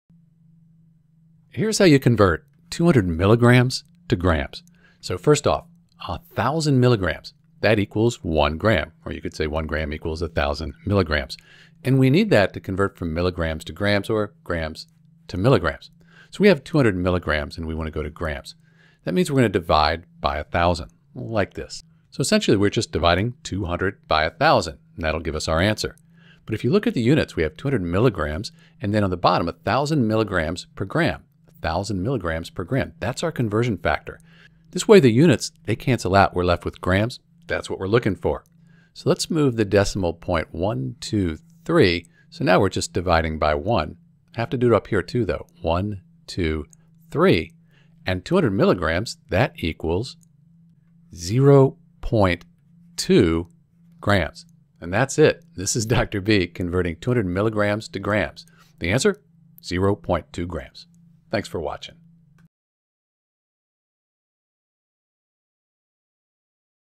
How to Convert 200 Milligrams to Grams (200mg to g)
To convert 200 milligrams to grams (200mg to g), you use the conversion factor that 1,000 milligrams = 1 gram. Since a gram is 1,000 times heavier than a milligram, you divide the number of milligrams by 1,000 to convert to grams.

Steps for conversion:

🔹Start with the amount in milligrams, which is 200 mg.
🔹Divide by 1,000 mg/g, the conversion factor, to find the number of grams.
🔹Conversion calculation: 200 mg divided by 1,000 equals 0.2 g.

When you divide, milligrams should cancel out and you should be left with grams.

So, 200 milligrams is equal to 0.2 grams. The key step is dividing by 1,000, using the conversion factor that relates milligrams to grams. If you set it up correctly, the milligrams will cancel out, leaving you with grams.